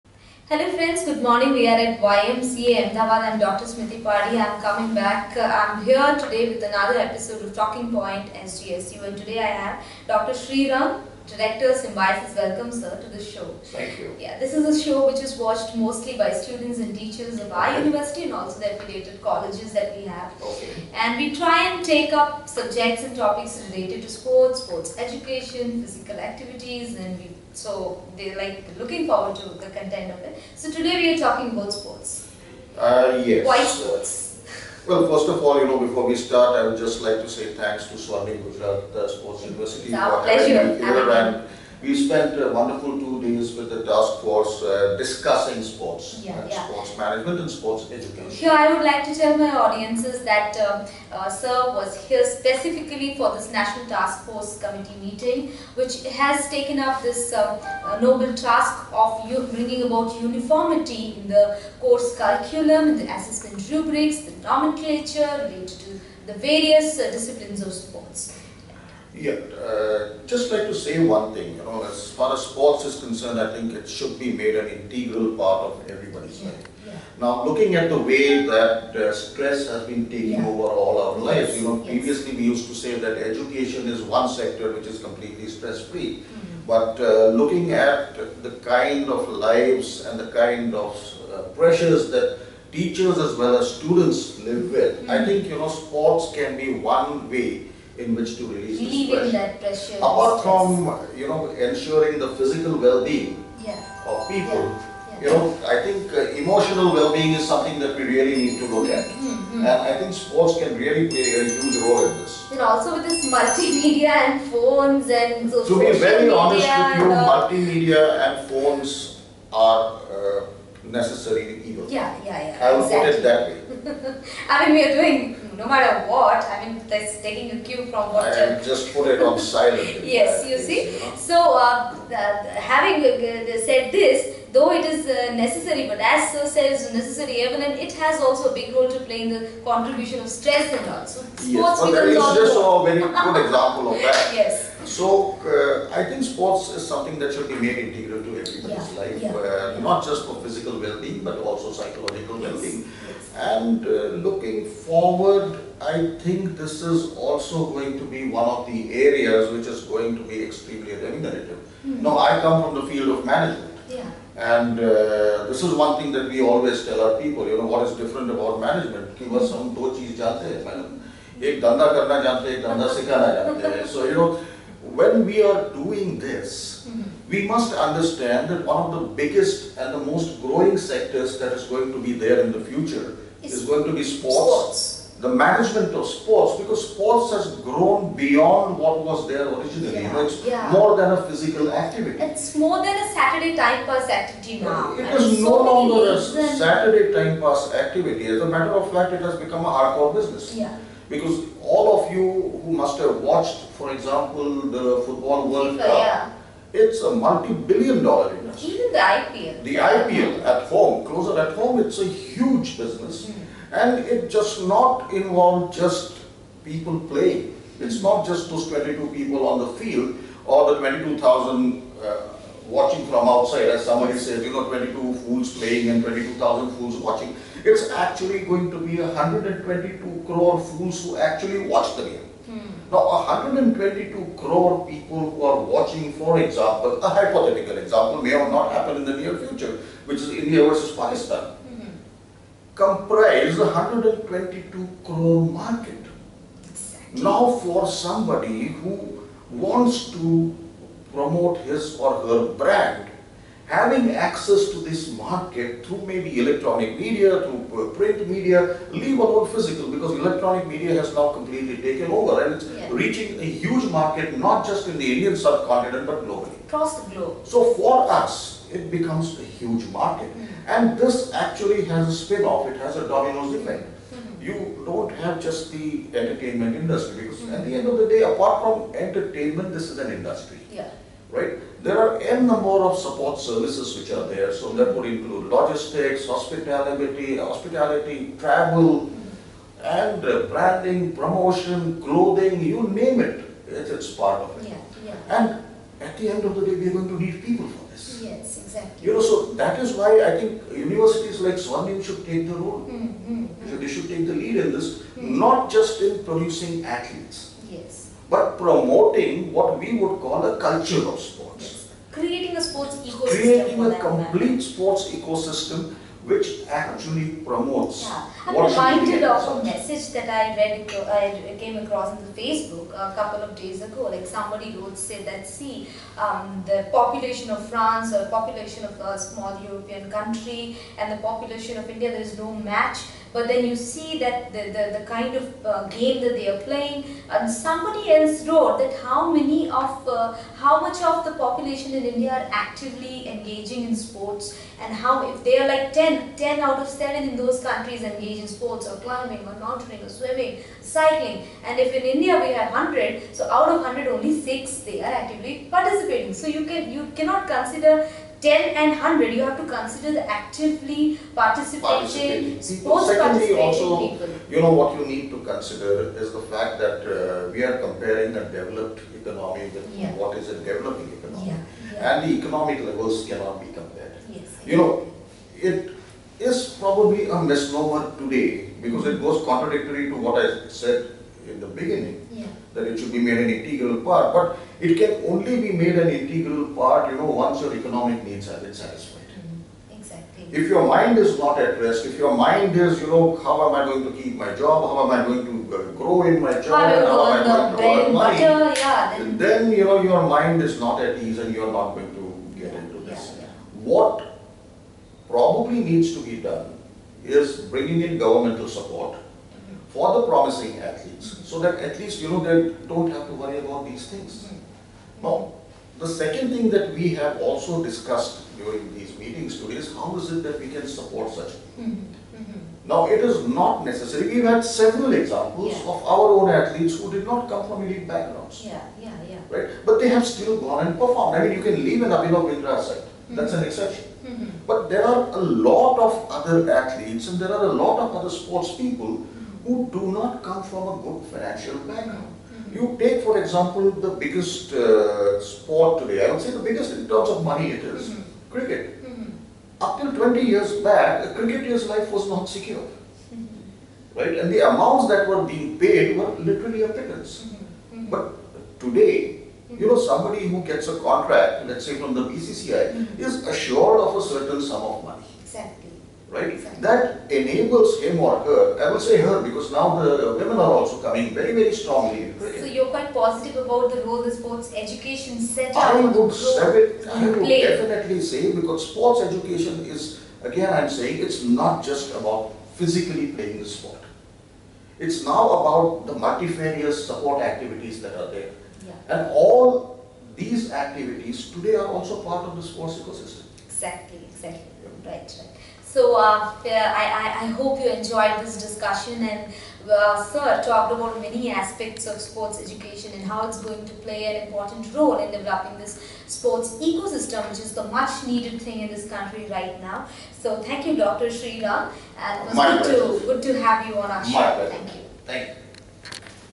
Talking Point - Swarnim Gujarat Sports University
Dr. Shrirang Altekar discussing the status and future of Sports Education in India